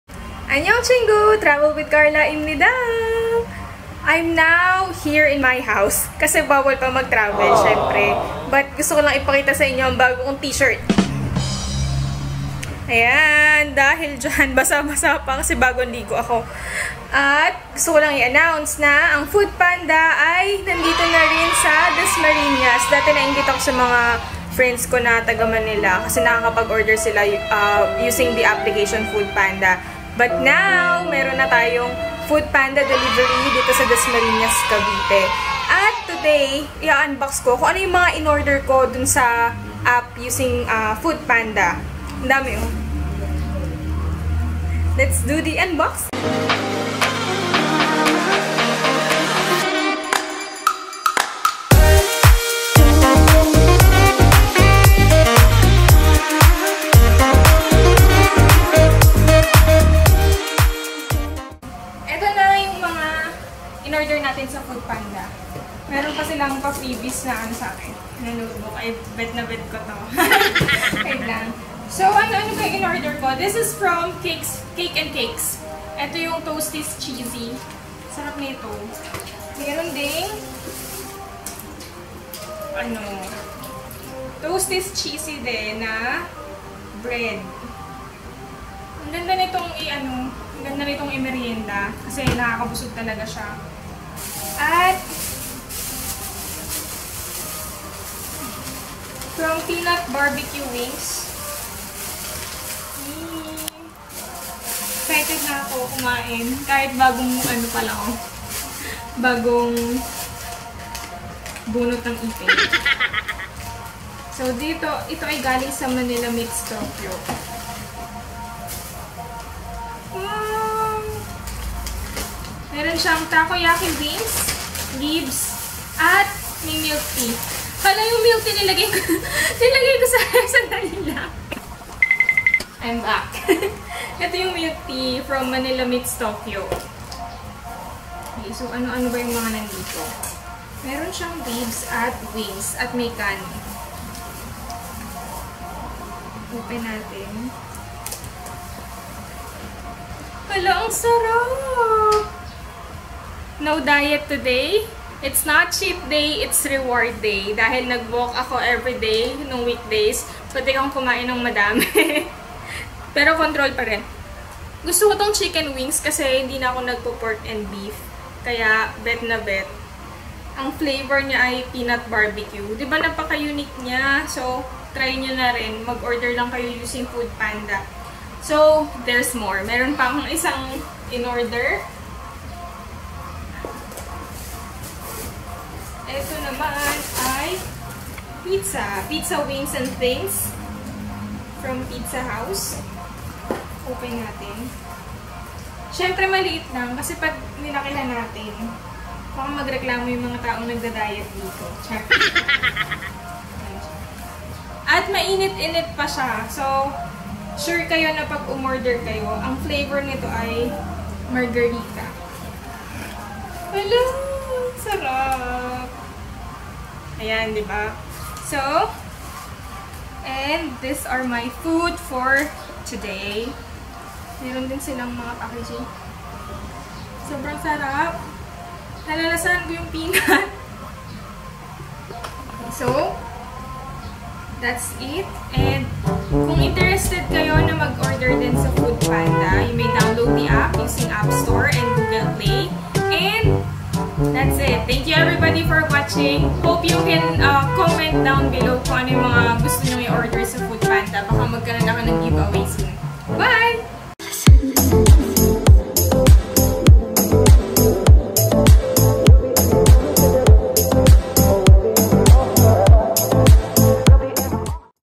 FoodPanda PH is now in Dasmarinas City!
So happy to announce that FoodPanda PH is now in Dasmarinas City, Cavite! Check out some of the meals available :)

Cake&Cakes | Manila meets Tokyo | Pizza Wings and Things